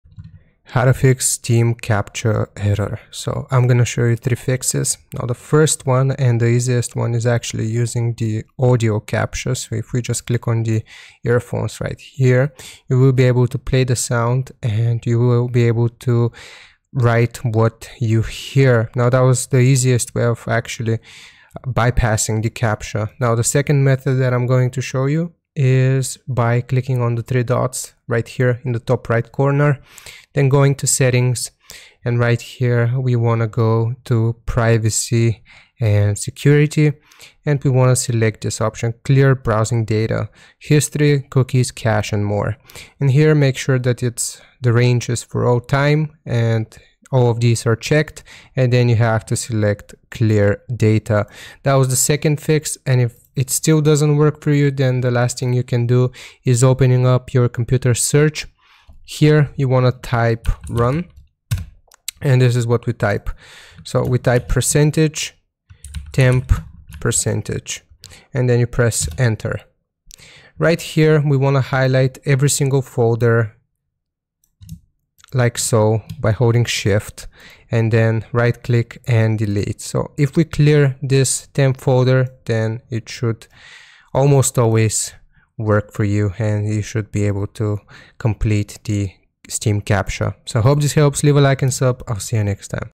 How To Fix Steam Captcha Error
In this video I show you how to fix Steam Captcha Error. If your Steam captcha isn't working then this is the tutorial for you.

Timestamps:
0:00 - Intro
0:07 - Fix 1
0:25 - Fix 2
0:55 - Fix 3